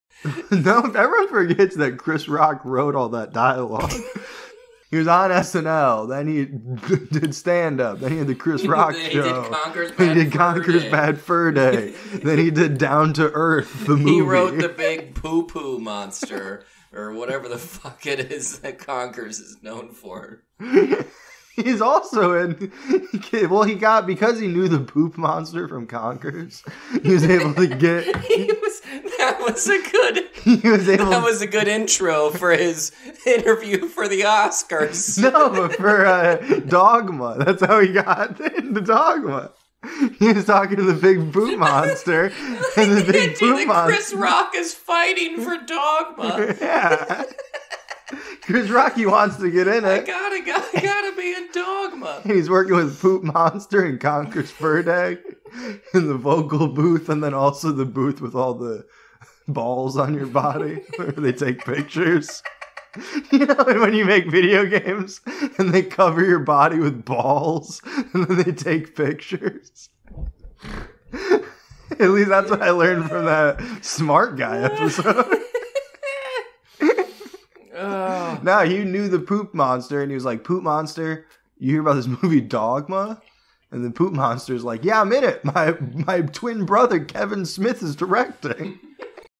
Funny Dinner | Chris Rock on the making of Conker's Bad Fur Day
Funny Dinner documentary about the making of Conker's Bad Fur Day and how Chris Rock wrote all the dialogue and also Dogma and Kevin Smith and the poop monster from Dogma and the poop monster from conkers bad fur day full episodes